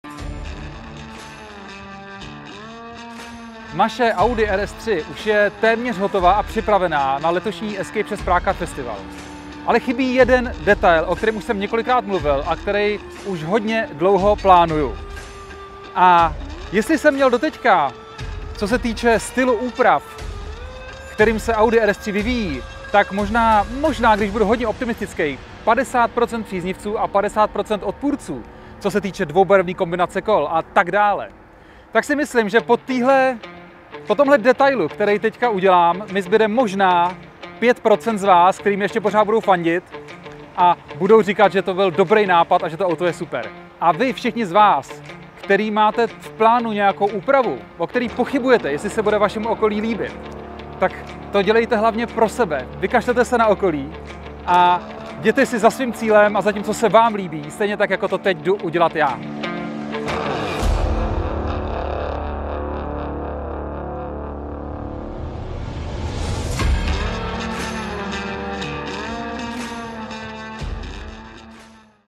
Escape6 | Audi RS3 | poslední detail před E6PCF
Že kombinace dvoubarevných kol je kontroverzní? A co tohle?
Uvidíte naživo na Escape6 Prague Car Festivalu 2019!